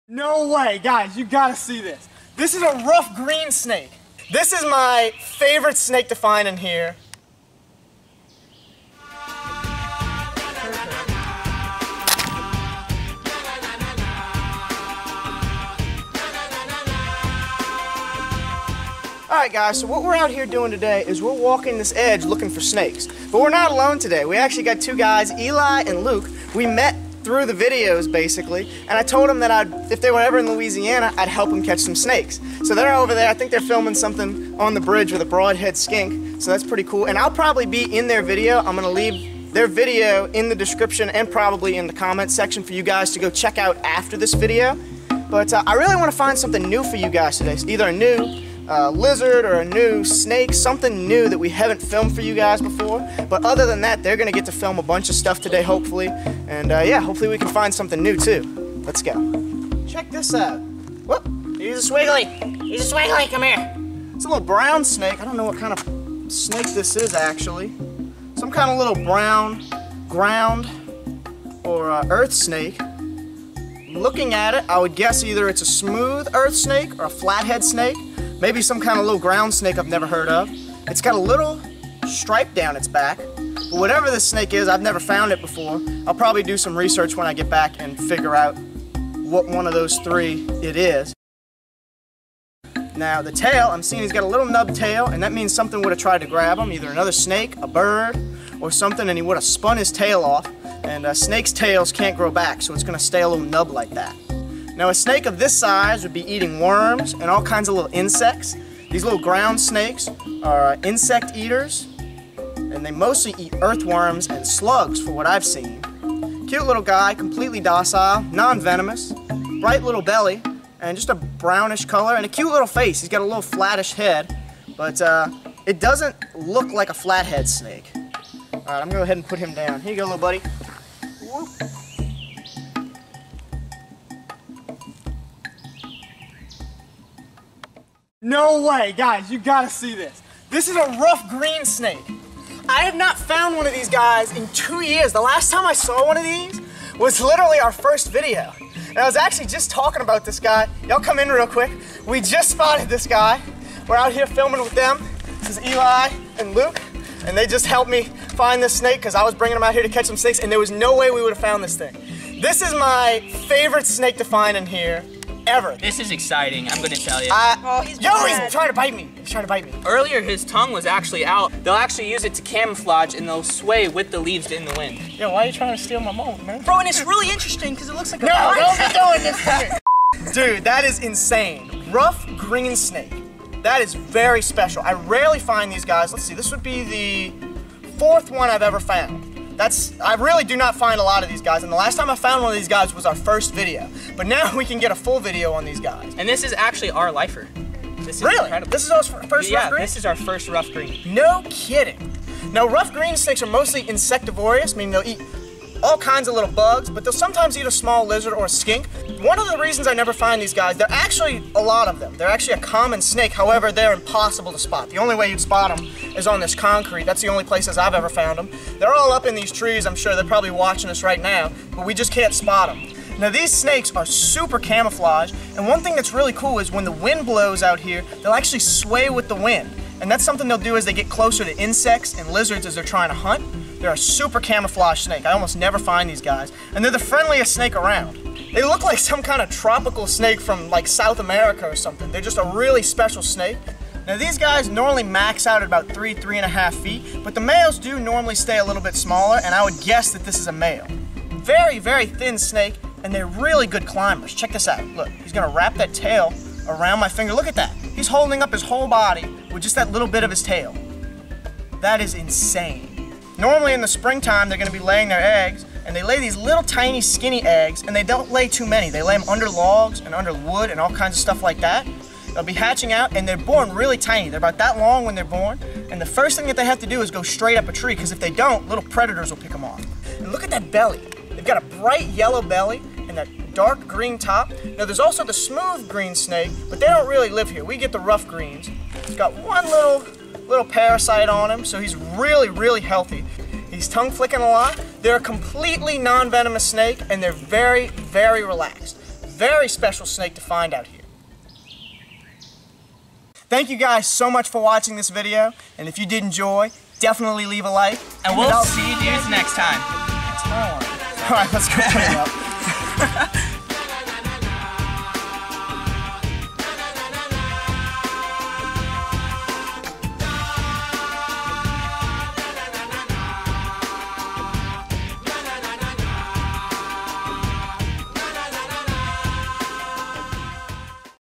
Adorable Green Snake!
On today's adventure, we get up close with one of the cutest and friendliest snakes in Louisiana! The rough green snake! Being a solid green, tree dwelling snake, means these guys are hard to find, and while out exploring with our guests, Eli and Luke, we found this adorable green snake!

Join Cajun Boy (Zachary Gray) as we learn about this super elusive, and super camouflage green snake! As always we hope y'all enjoy this video! :D

Go check out Eli and Luke's video to see more of the adventure!

Most tropical or tree snake people think about, would probably look exactly like this snake. A bright green color, a yellowish belly, and a think, stick like build made for climbing and hunting in the trees. This arboreal species of tree snake, while not endangered or vulnerable,  is rarely seen by people, because of their camouflage and tendency to stay high in the trees. Green snakes mostly prey upon insects, but will also eat other reptiles, such as lizards. One thing is for sure, these adorable snakes are a nonvenomous, and completely harmless species. So if you ever see one, definitely don't worry. These guys are friendly and cute!